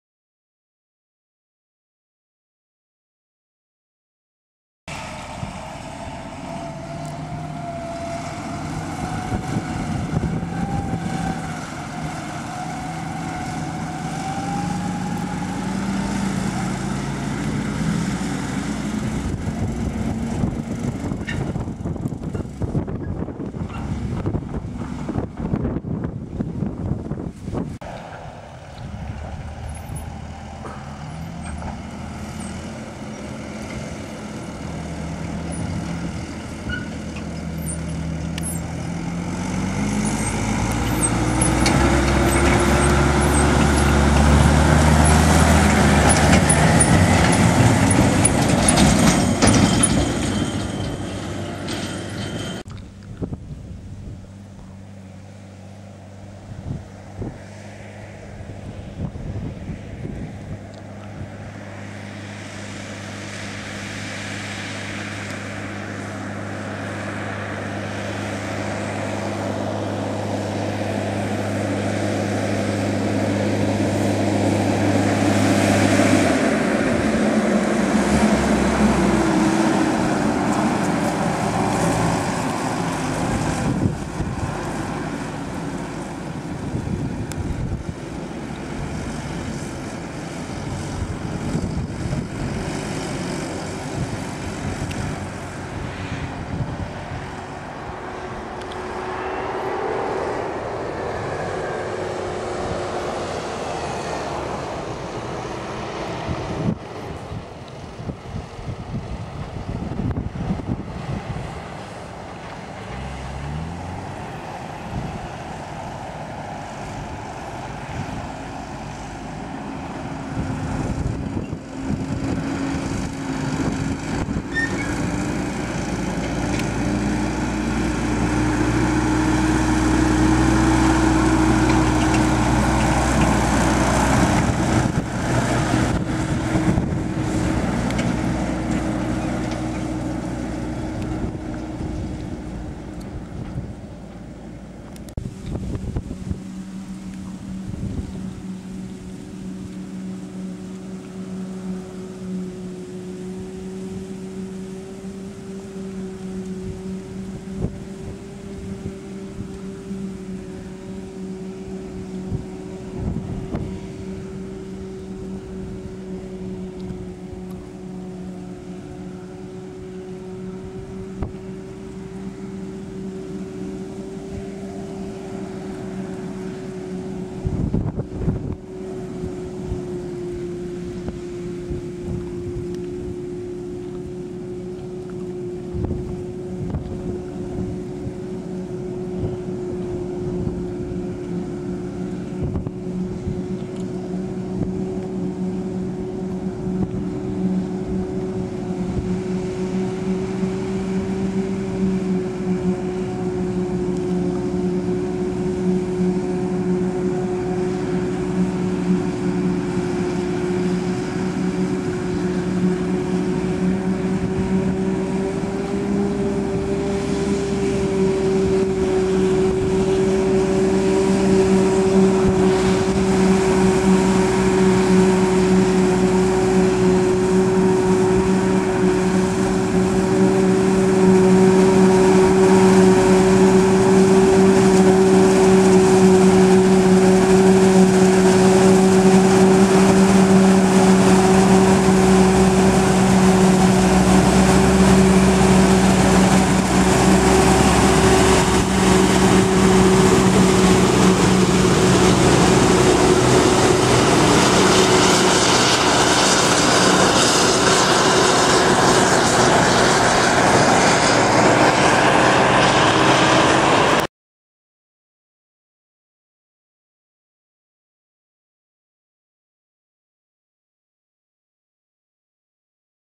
Maize 2010 Keat Contractors at Wills Bros Ltd, Part 2.wmv
Maize 2010, Keat Contractors at Wills Bros in Cornwall. Claas Jag 890, Fendt 924 with Rolland trailer & 3 x Fendt 718 with Marston 11t trailers.